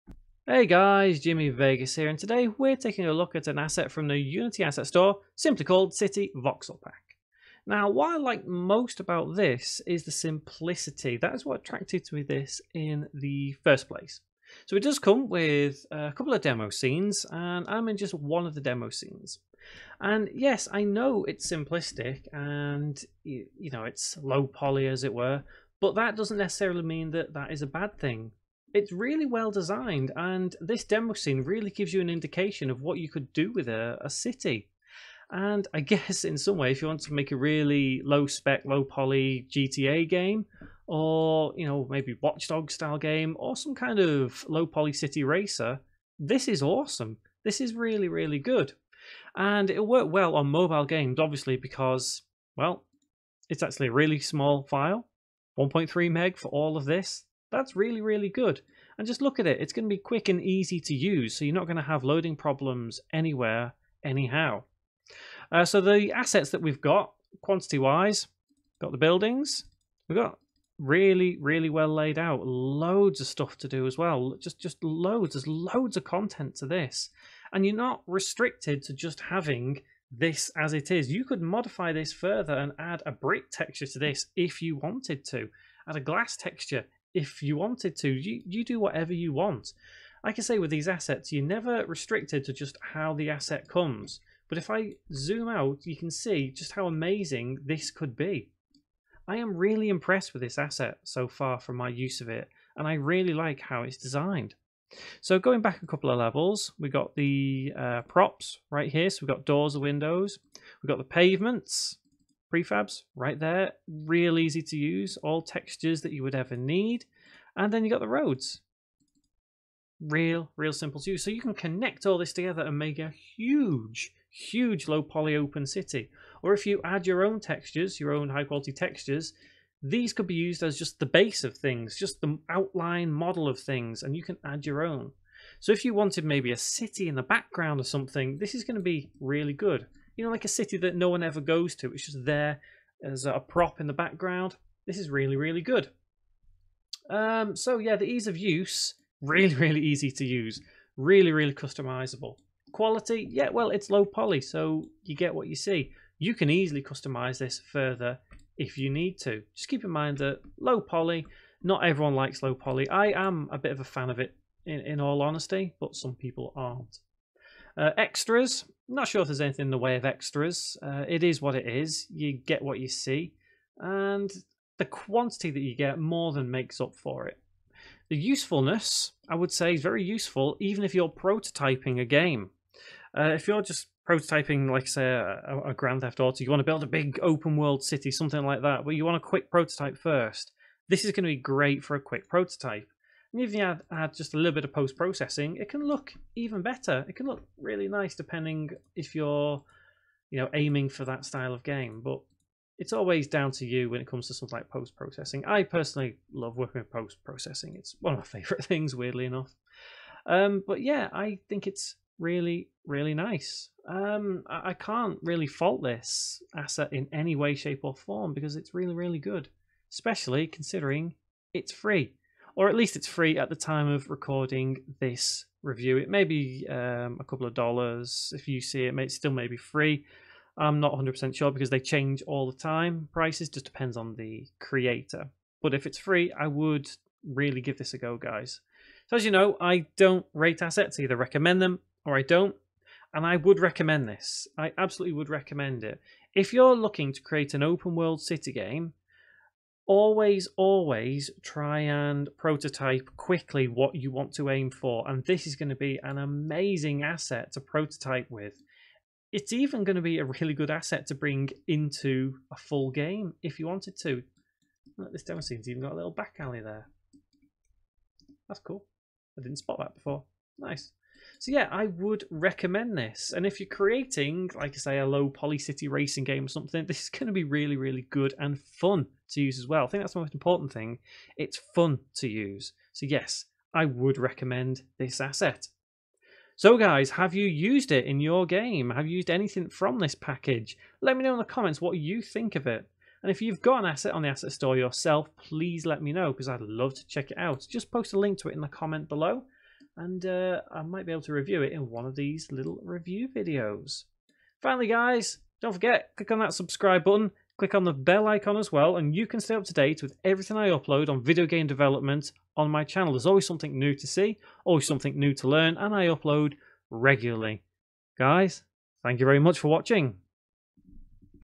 UNITY ASSET REVIEW | CITY VOXEL PACK | INDEPENDENT REVIEW BY JIMMY VEGAS ASSET STORE
A free package with a lot of different voxel buildings and roads for your cities. Review conducted independently and no money was received for this review.

Who Is Jimmy Vegas?